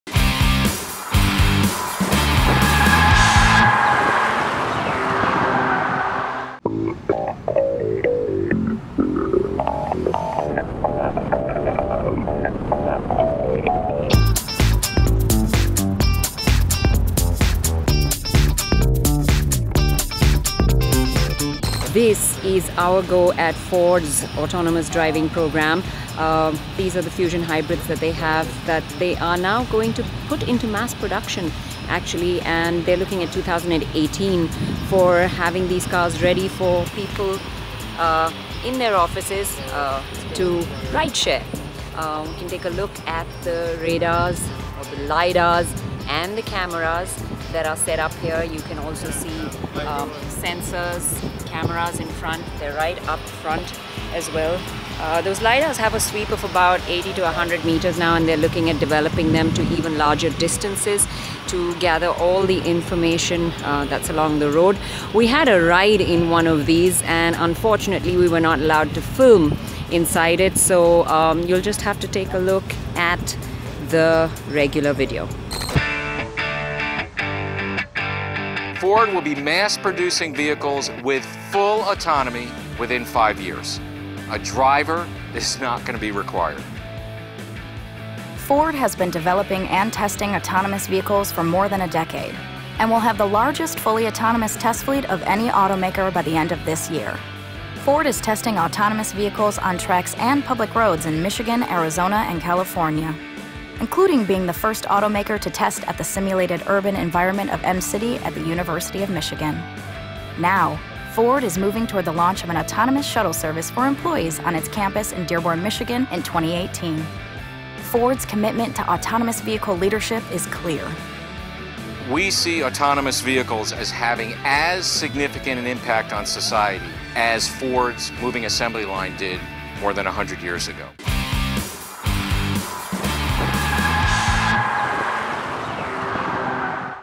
Ford Fusion Autonomous Vehicle | First Look | Autocar India
An autonomous vehicle service for Ford employees will be available on Ford's Dearborn campus in 2018.